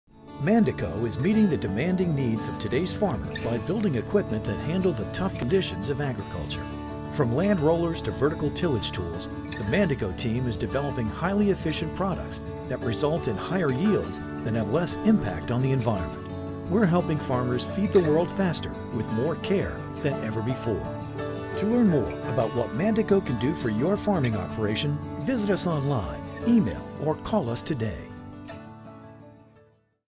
Mandako TV Commercial
From landrollers to tillage tools, Mandako is determined to provide the agriculture world with exceptional farm equipment that will promote business growth, and in turn provide more resources for the ever growing world as a whole.